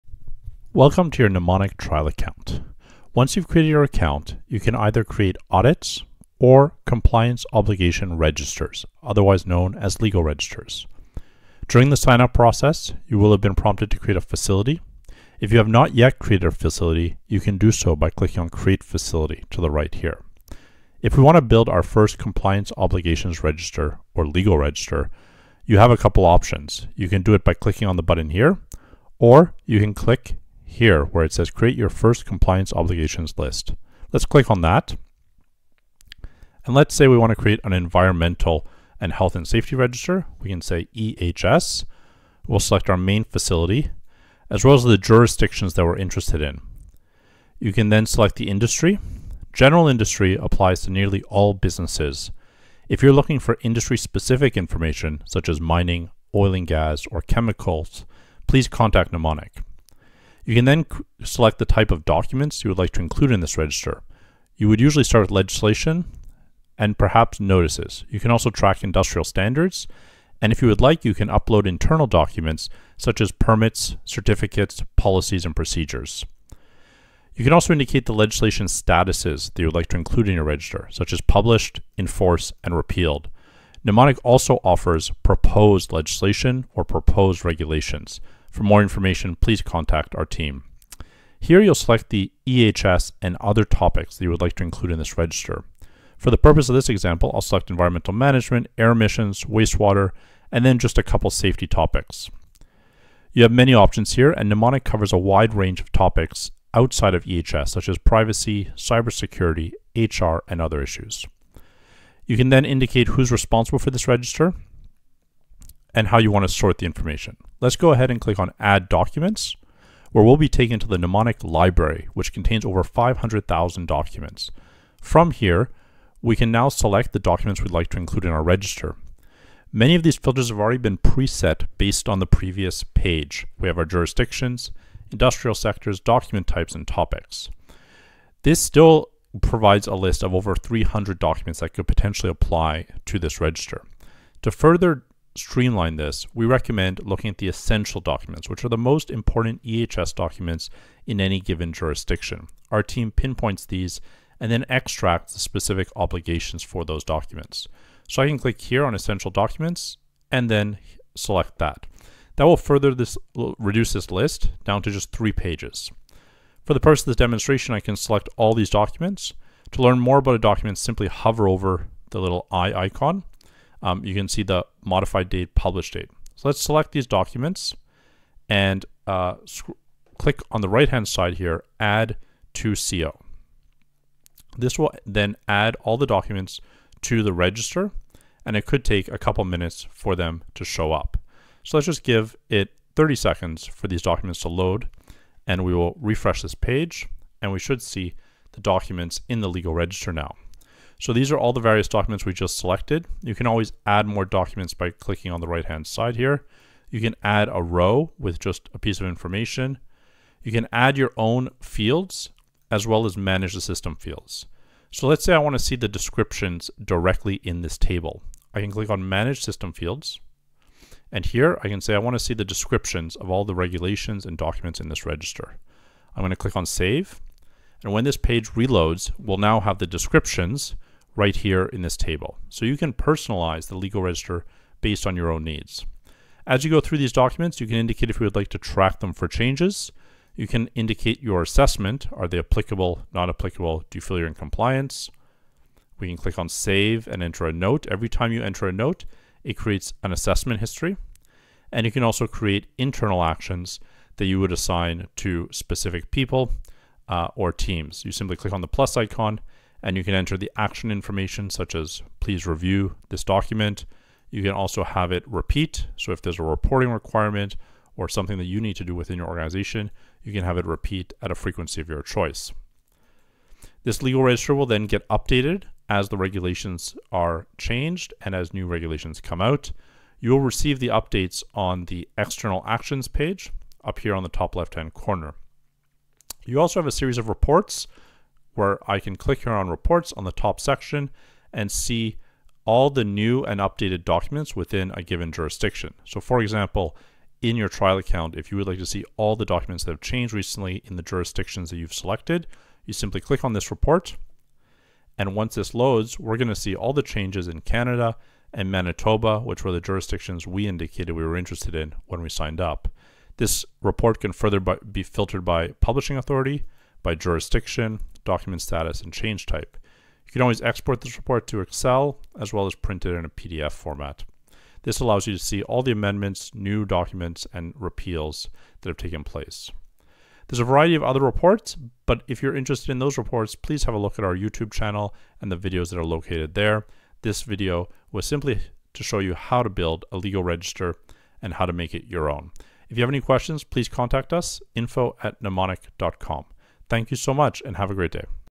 Create an ISO Legal Register / Compliance Obligations in Seconds
Build a legal register or compliance obligations register in seconds with Nimonik. Simply create a trial account, select relevant legislation and start annotating and assigning responsibilities. This short video demonstrates how to build a legal register and receive updates about important environmental, health and safety legislation in the jurisdictions you operate in.